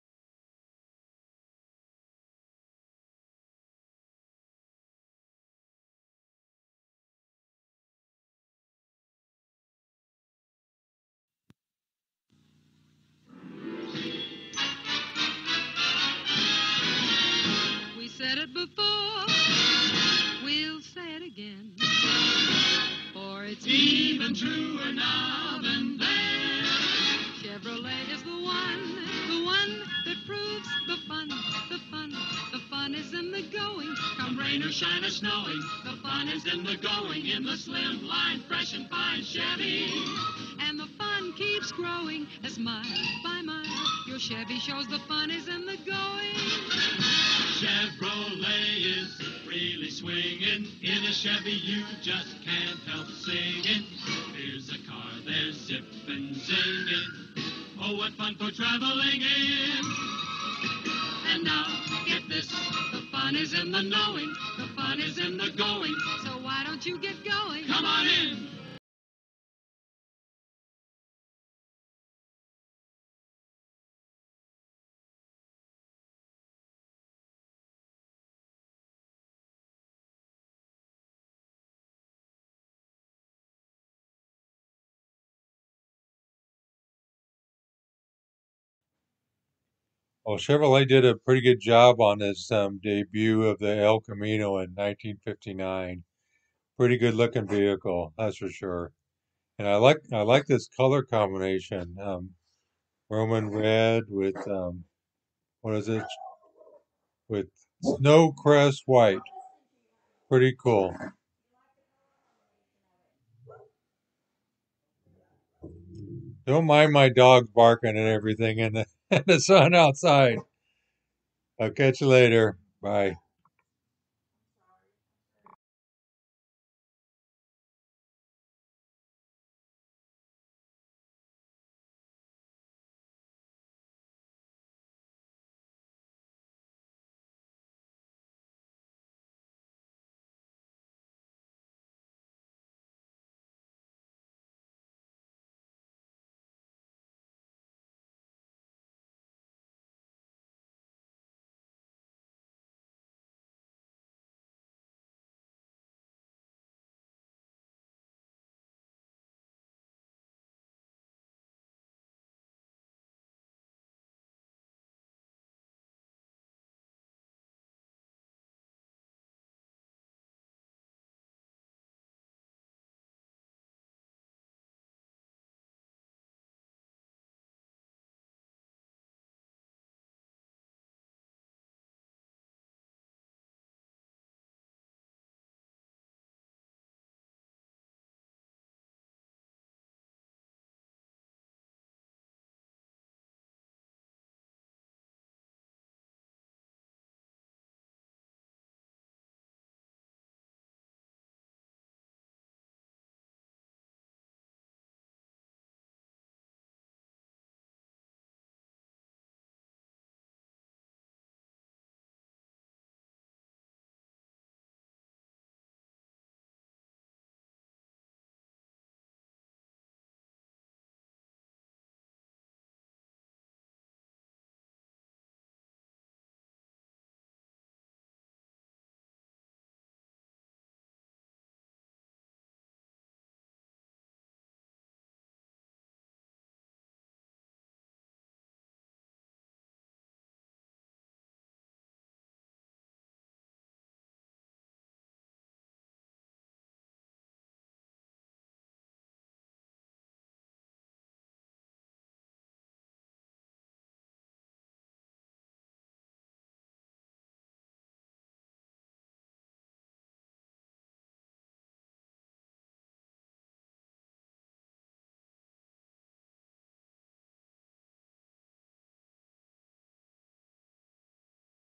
59 Chevy El Camino + AMX brochure
A good looking, well thought out first entry...the 1959 Chevy El Camino,

Also a brochure and adds back in the day for the AMX...hope you enjoy...Dave/WBA